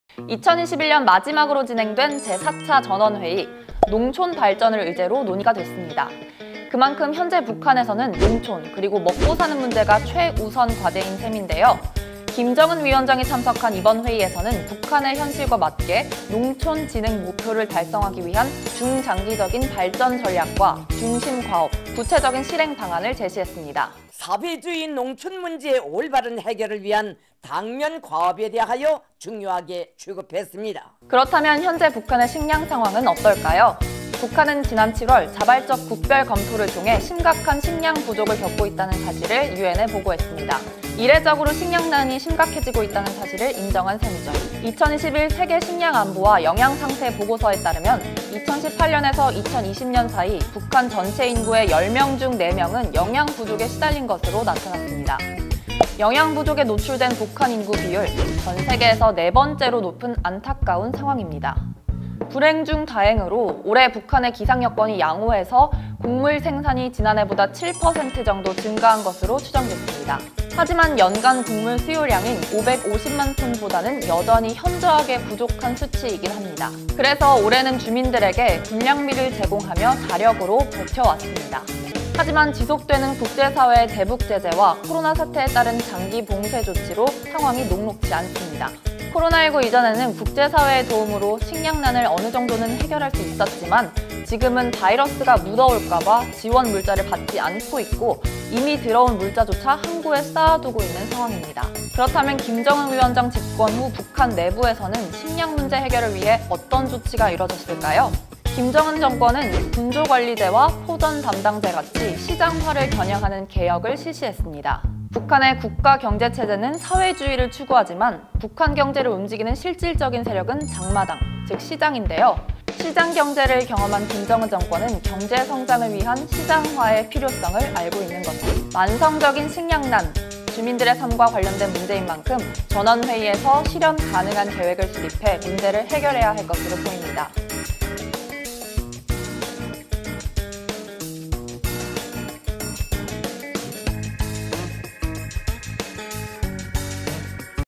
김정은이 자꾸 살빠지는 이유!? 식량난 ?! 🌾 | 2021년 북한 전원회의 핵심은?
2021년 마지막으로 진행된 제4차 전원회의
이번 회의의 핵심은 ‘식량문제’였습니다.

그만큼 현재 북한에서는 농촌문제와 먹고사는 문제가 최우선 과제인 셈인데요.

오늘 한반도N에서는 북한이 이 문제에 대해
어떤 목표와 해결책을 마련했는지 알아보려고 합니다!

프로듀서 김효원 / 촬영 김정용 / 크리에이터 신재은